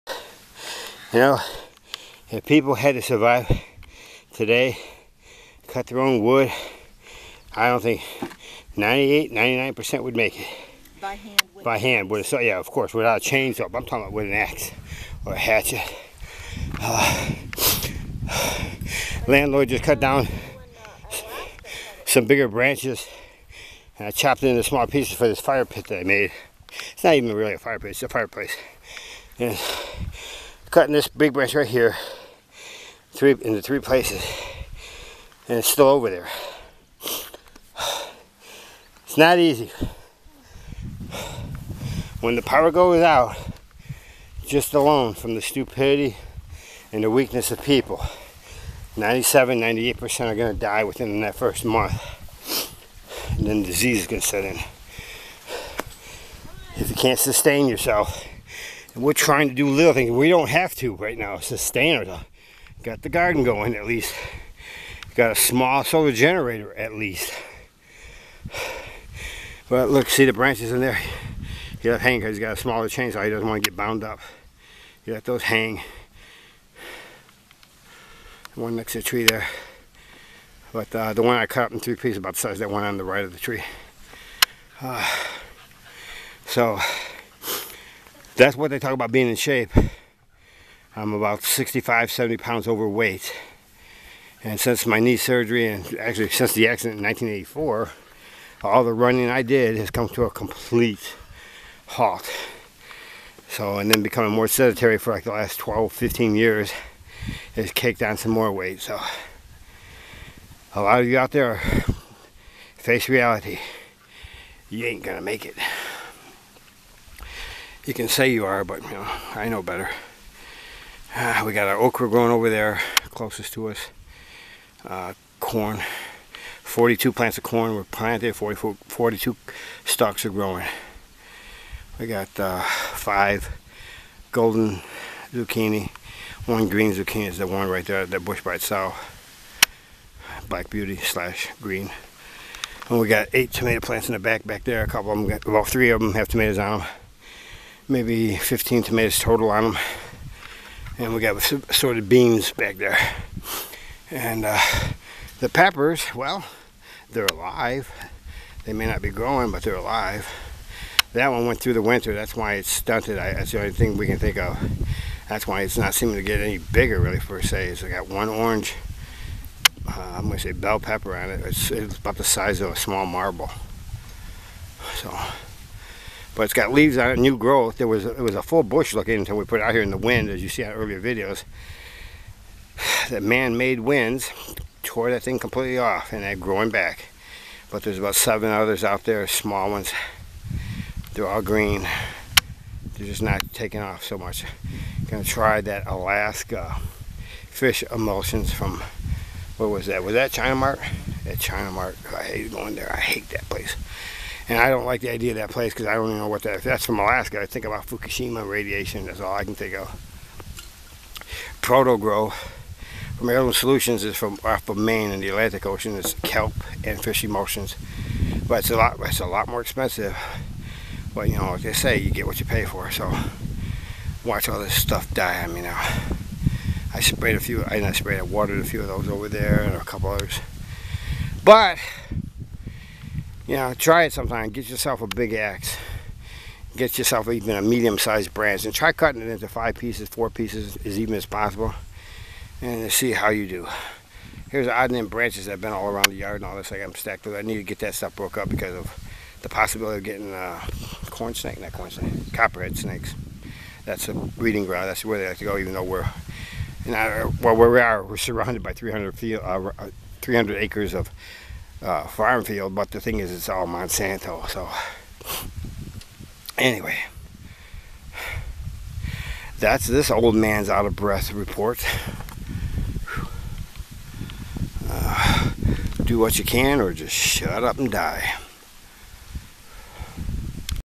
CAN YOU make it 2016 06 08
Do you "feel"... you can MAKE IT? What our Great Grand-Parents did...99% of  YOU could NOT HANDLE! I simply used an axe to chop a SMALL branch in 3 parts and  IT IS "NOT" as easy as many of you DON'T... know.